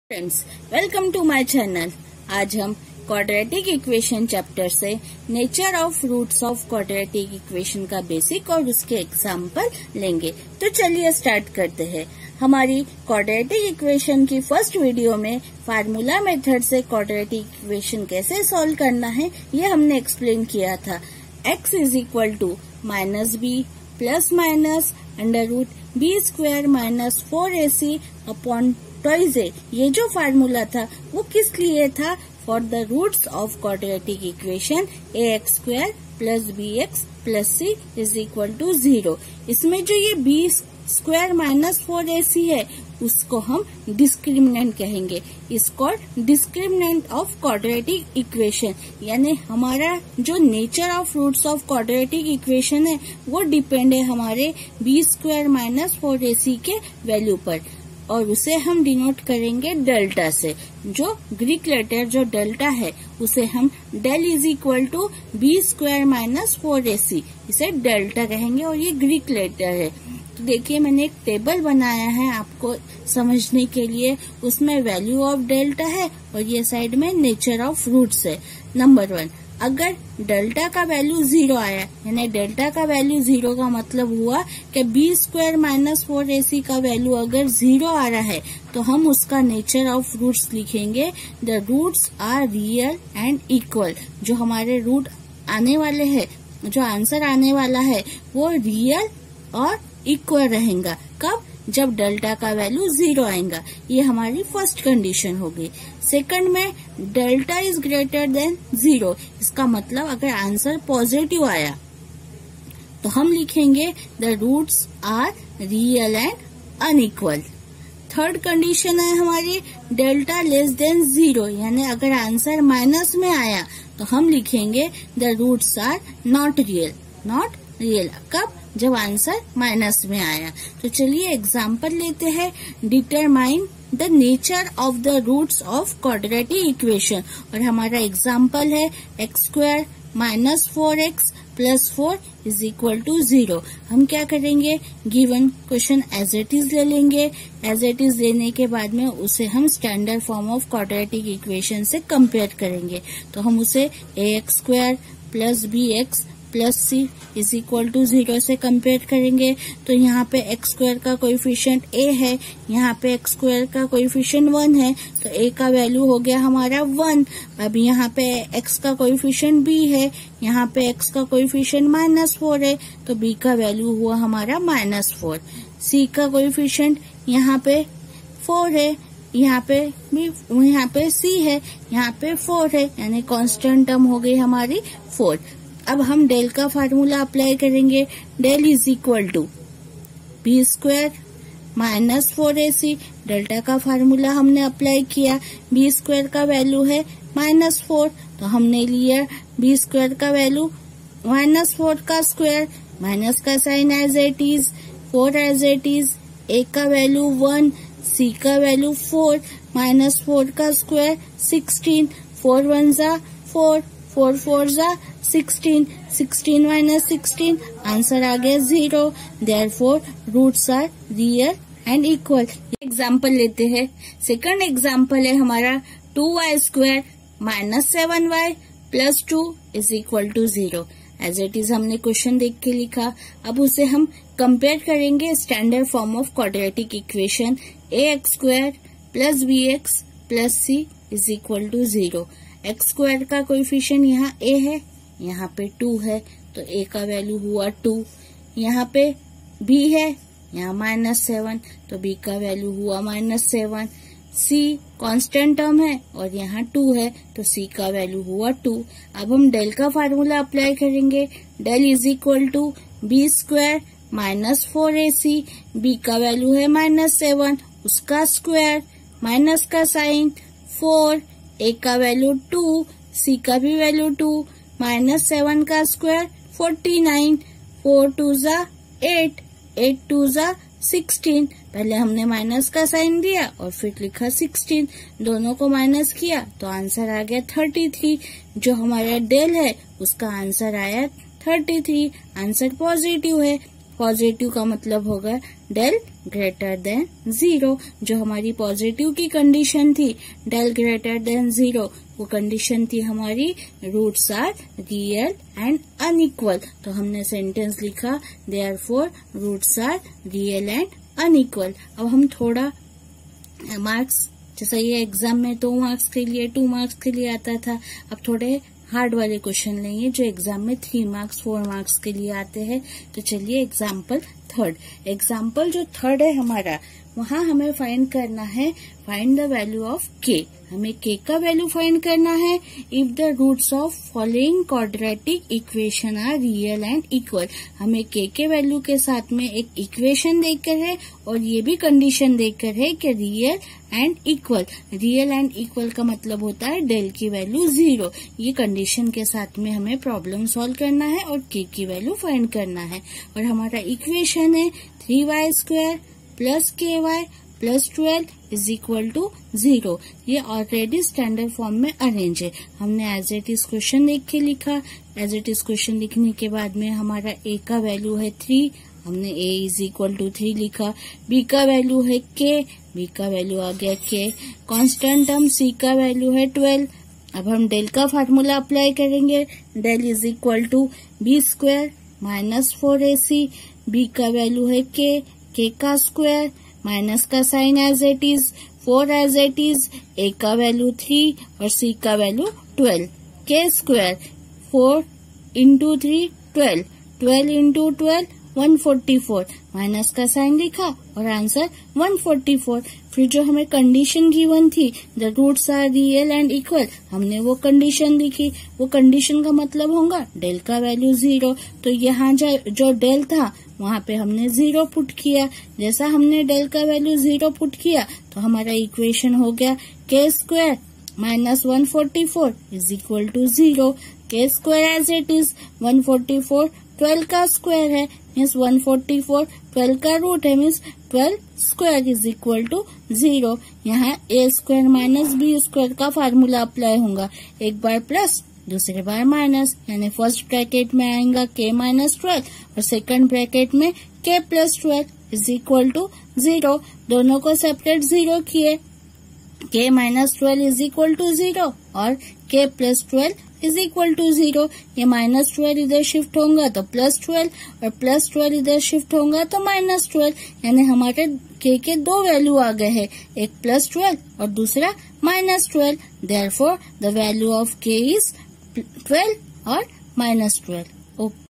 Nature of the Roots of Quadratic Equations। Class 10th। Nature of the Roots and examples
Nature of the Roots of Quadratic Equations
 Class 10th
Nature of the Roots and examples

Aaj ki hamare video me hum Quadratic Equations chapter se Nature of the Roots of Quadratic Equations quadratic ka basic and example easy method se explain karenge.